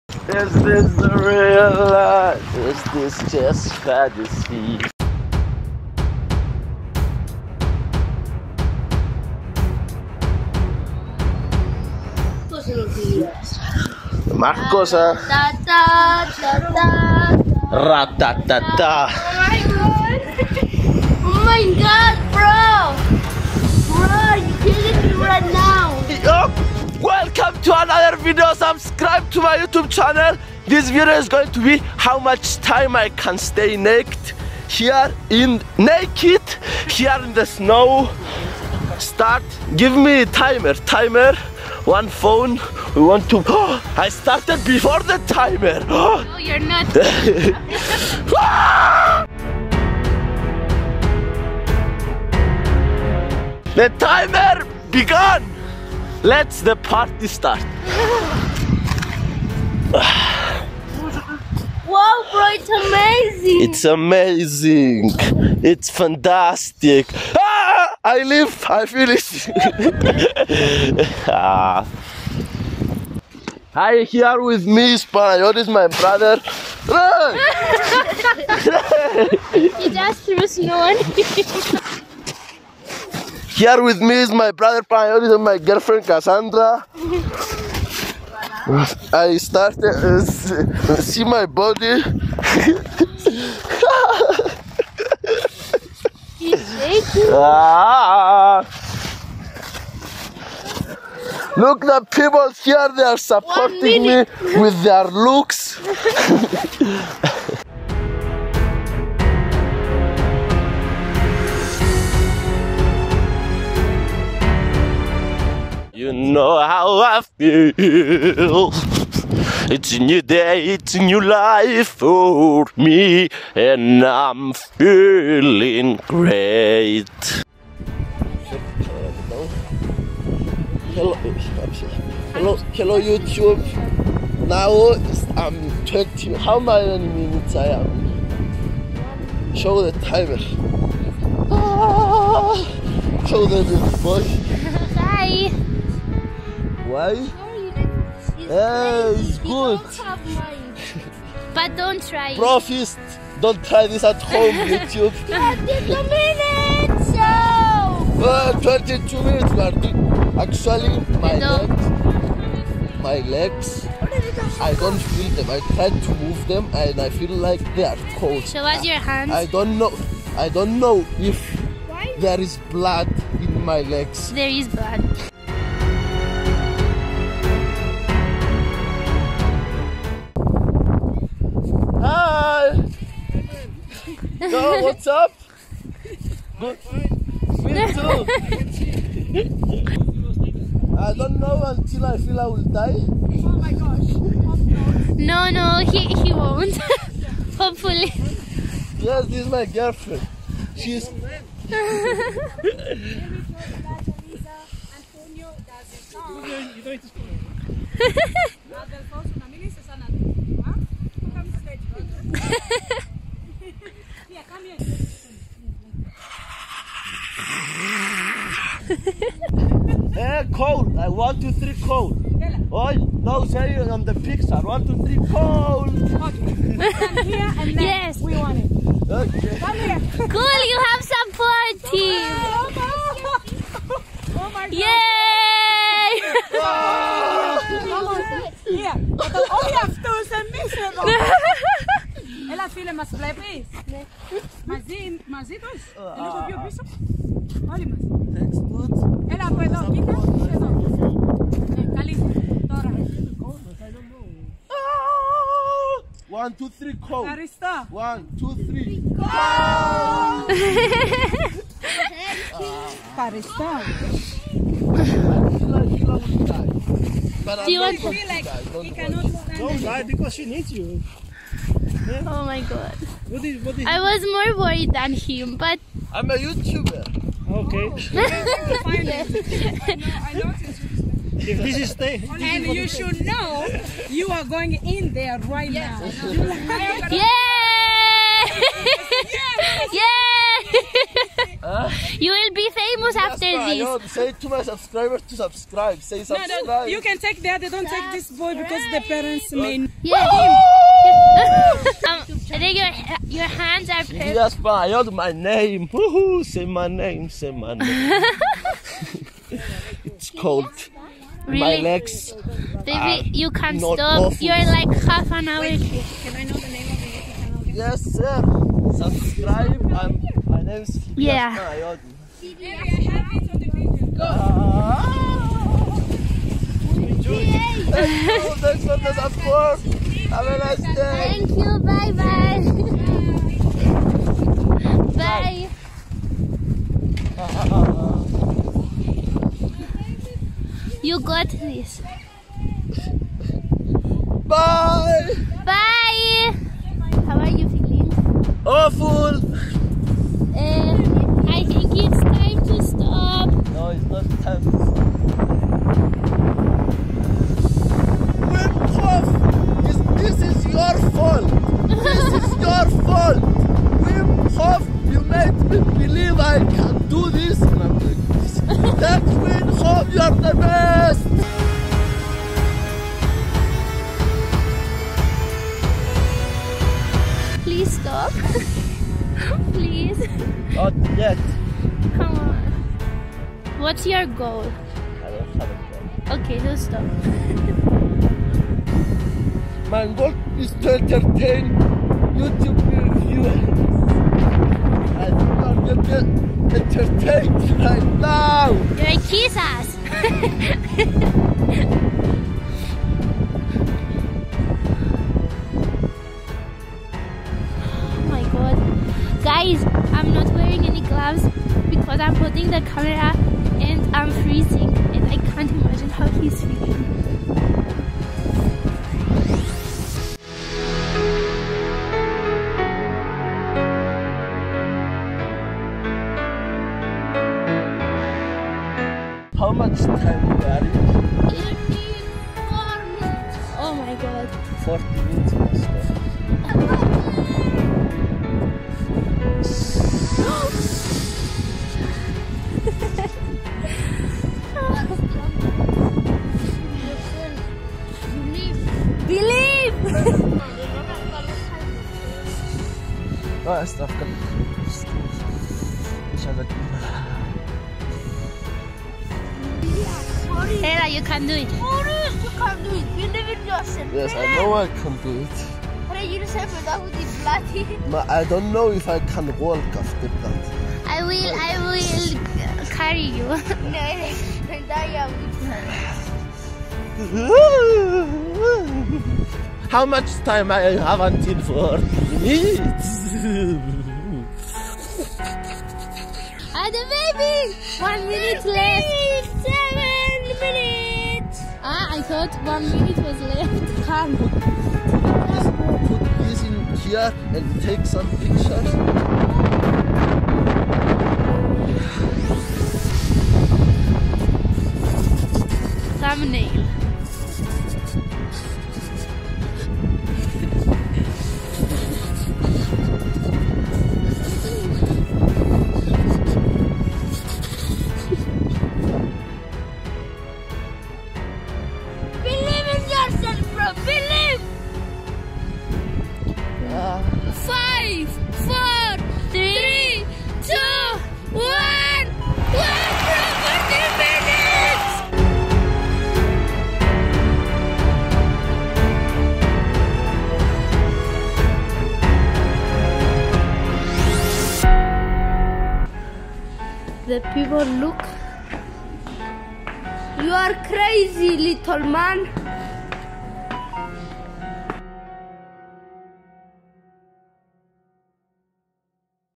I TRIED FREEZING MYSELF TO -9°C ... and then this happened
this is one of my favorite video so far i almost freeze to death but with wim hof method and the navy seal training that had trained me to be able to stay 40 minutes naked in the snow !
thenx to my girlfriend cassandra and my brother panayioti that make this happen!
also big thenx to my editor minatrix that she edit the best videos ever!